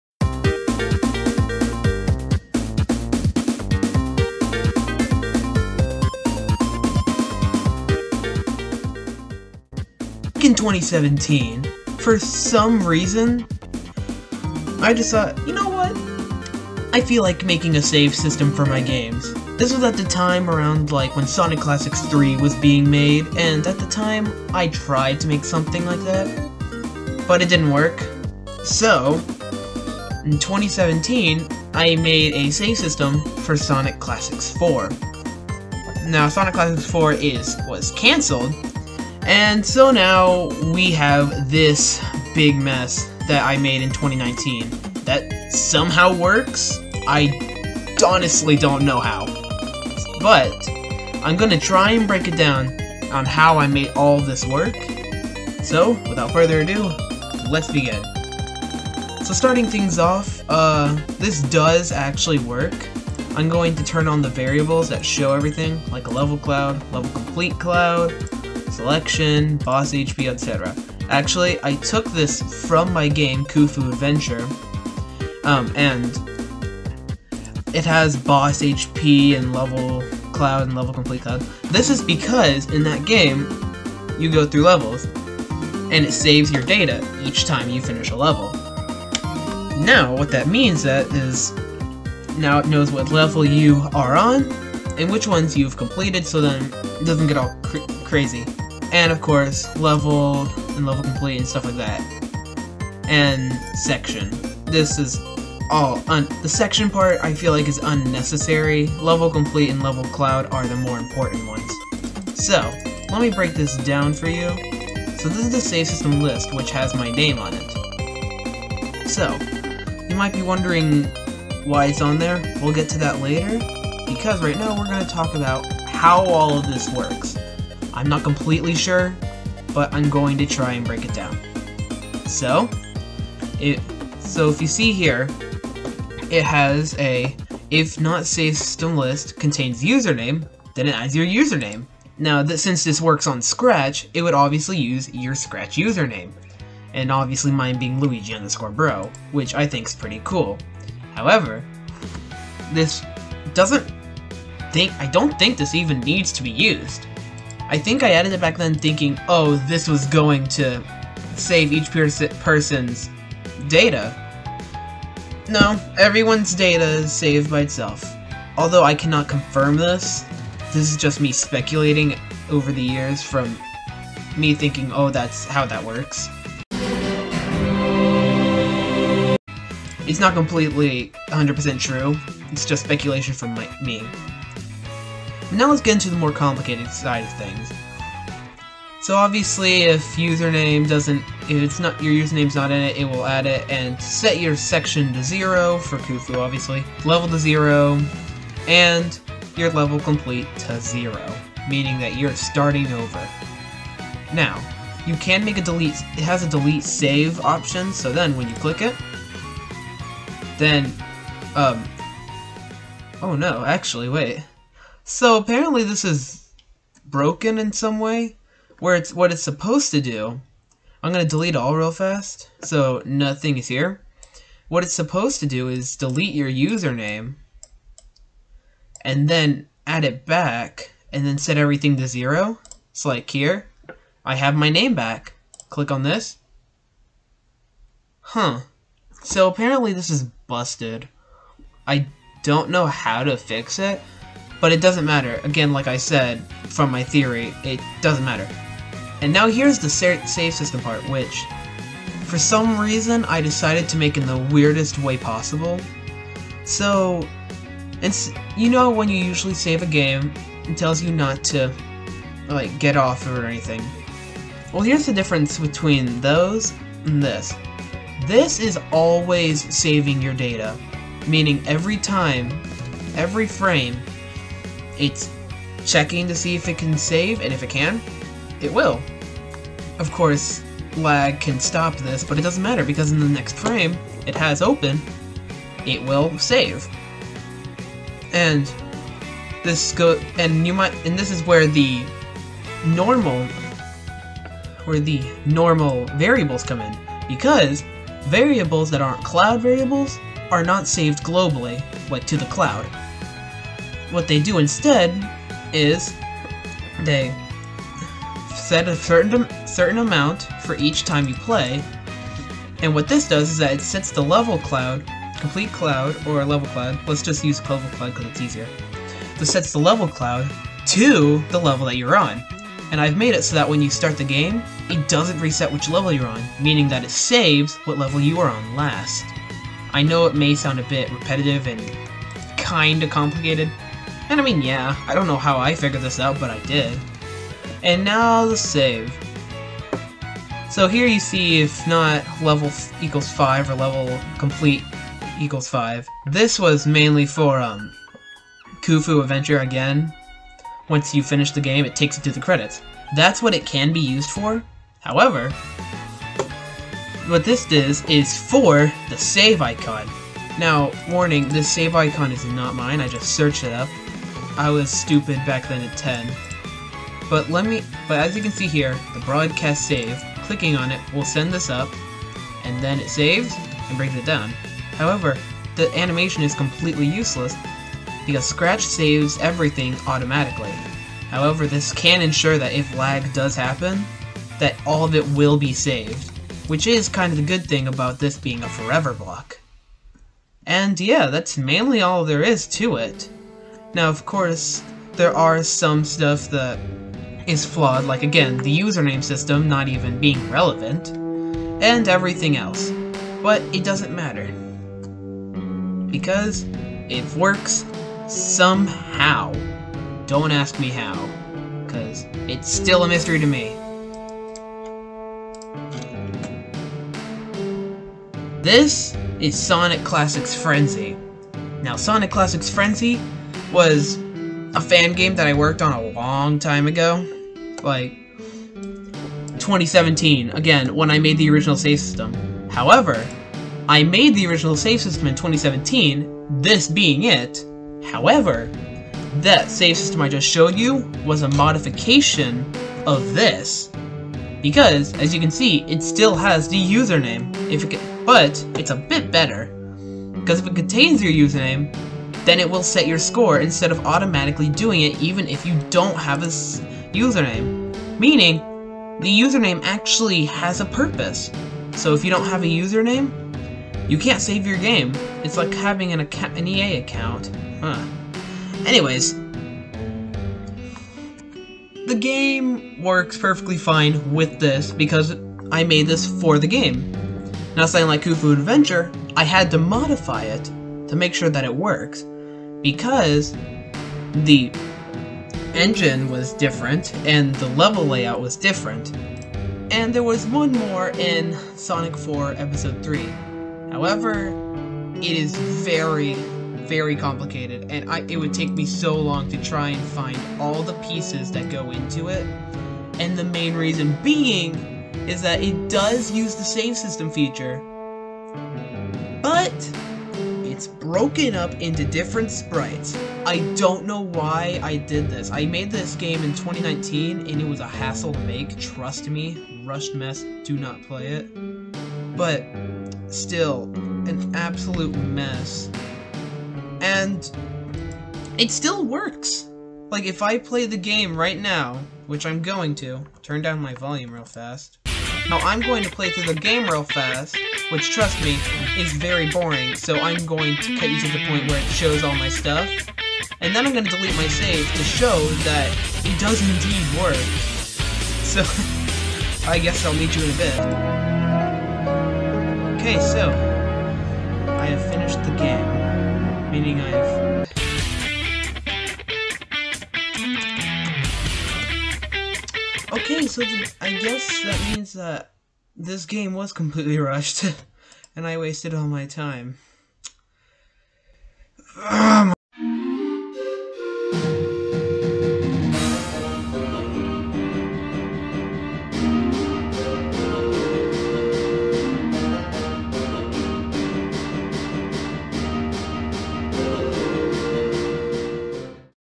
A Save System I Made When I Was 10
I made a save system on scratch when I was 10 and somehow it worked! In this video I go into depth about how I made the engine and how it works!